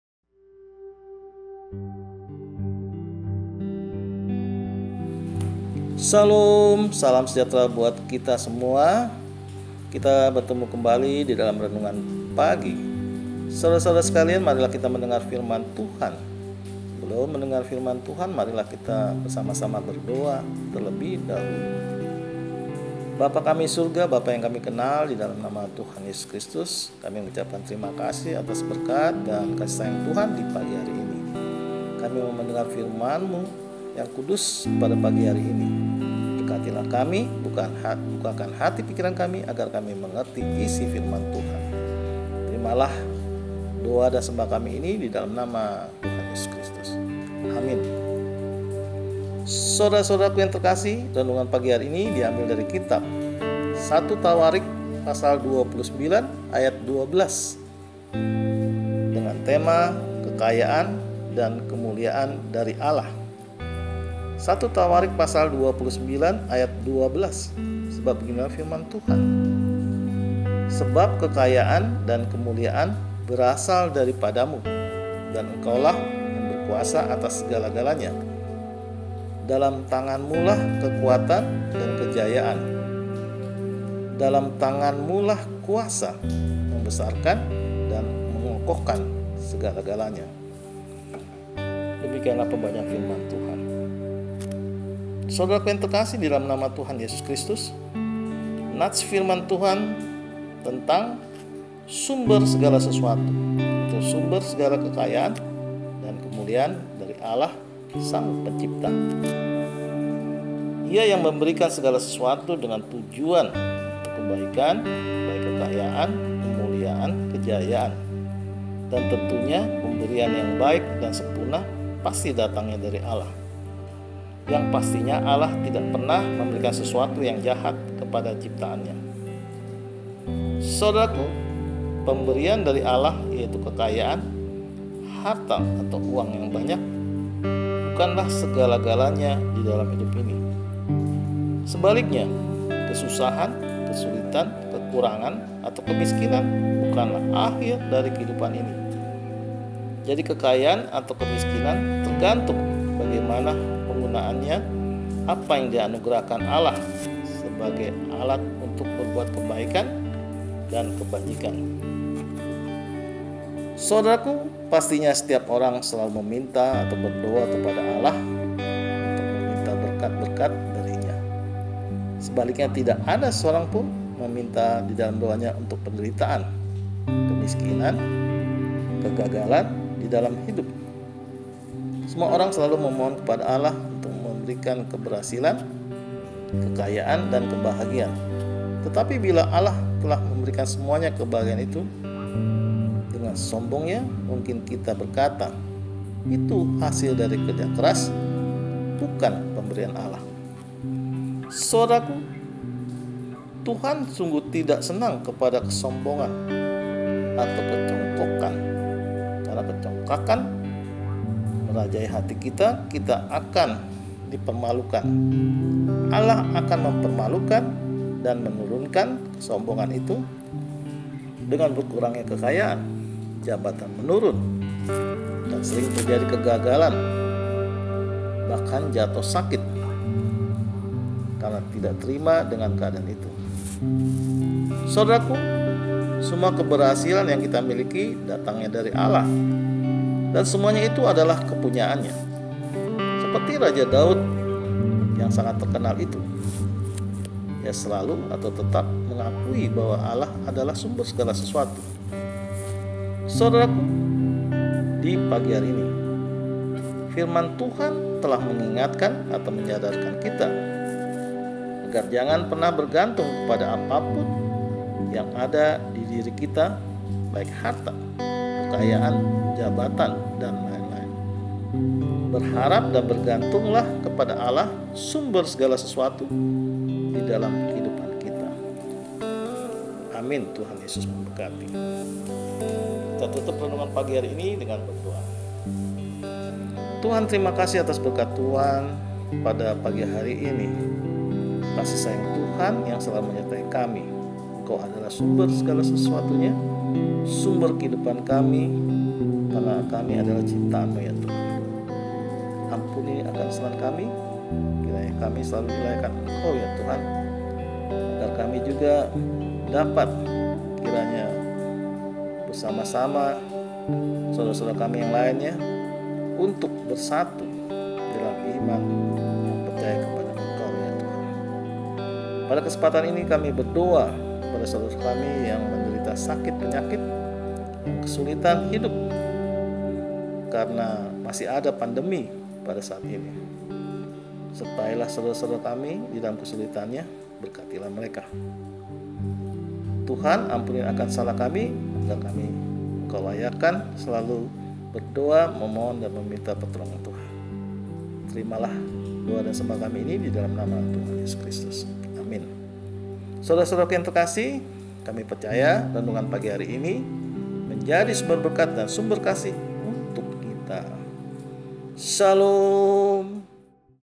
Kekayaan Dan Kemuliaan Dari Allah
Renungan pagi ini diambil dari kitab 1 Tawarikh 29: 12